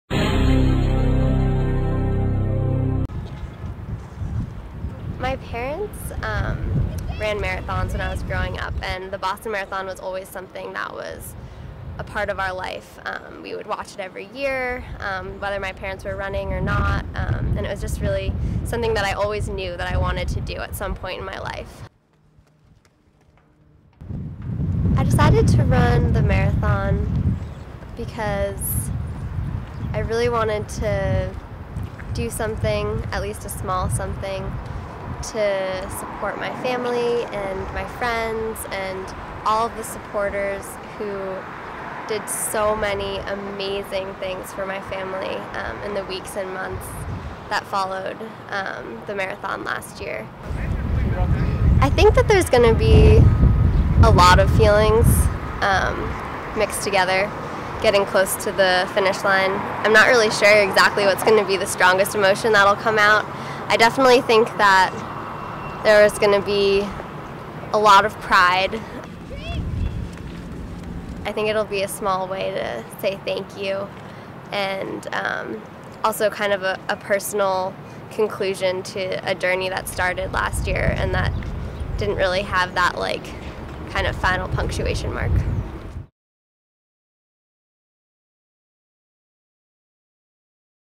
Danielle Reny Runs Boston Marathon for Sister
College student Danielle Reny trains to run the 2014 Boston Marathon for her sister who was injured in the bombing last year.